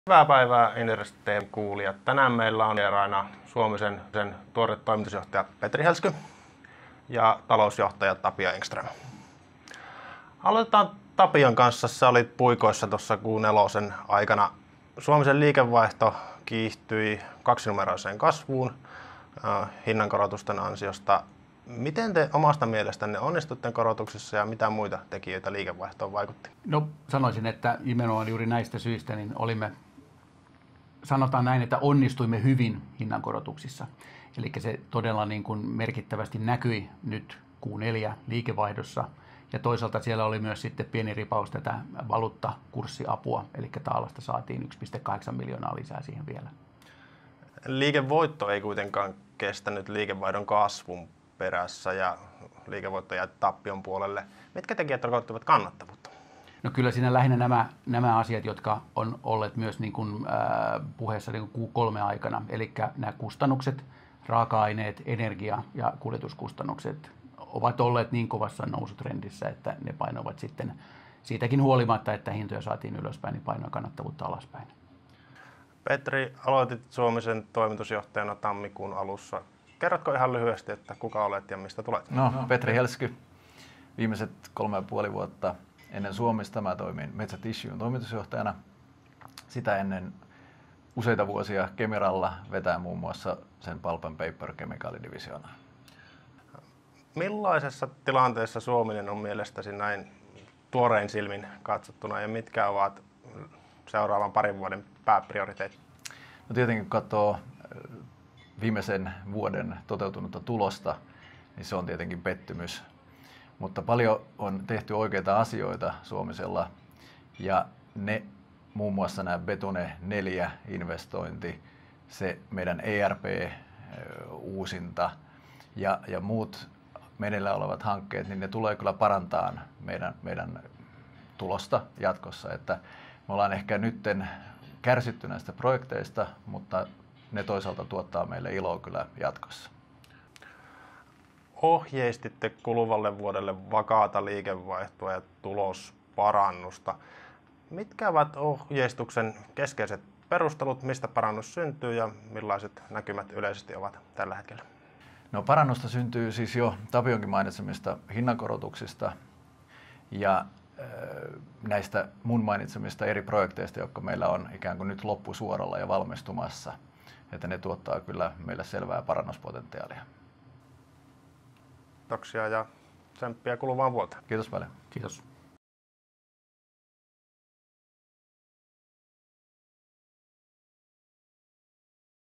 Suominen tähtää tulosparannukseen
Suomisen toimitusjohtaja Petri Helsky ja talousjohtaja Tapio Engström analyytikko Antti Viljakaisen haastattelussa vuoden 2018 sujumisesta ja millä rakennuspalikoilla Suominen tähtää tulosparannukseen 2019?